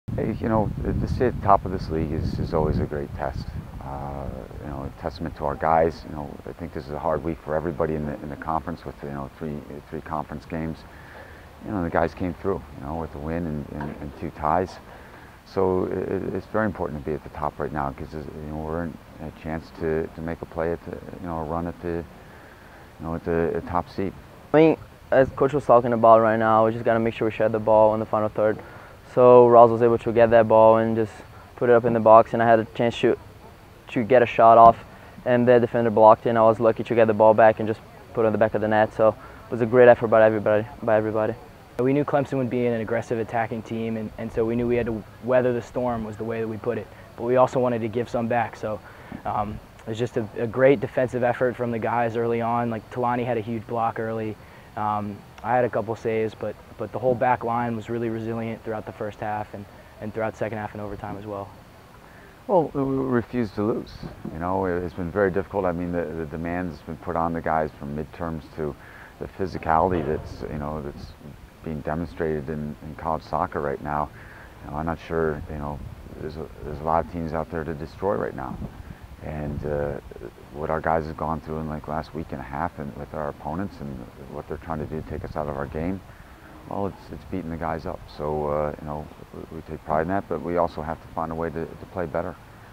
Wake Forest vs. Clemson - Postgame Oct. 11, 2013
Wake Forest and Clemson tied in men's soccer 1-1 on October 11, 2013 at Spry Stadium. Luca Gimenez scored for the Demon Deacons.